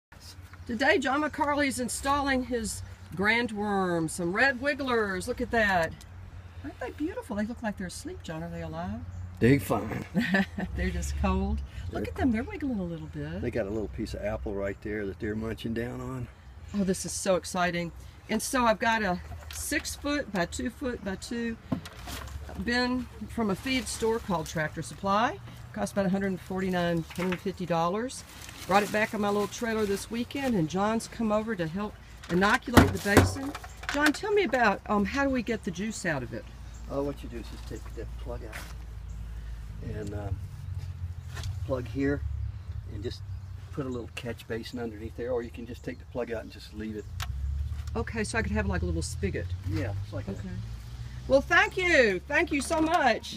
Grandworms! Dovecote's vermiculture bed
Today, John McCarley, guru horticulturist, came over with a bucket of beautiful red wigglers… Watch as we install them and get ready for a highly productive season.

Viva la permaculture... Dovecote hosts workshops for land and LifeScape. Be nourished so you can flourish!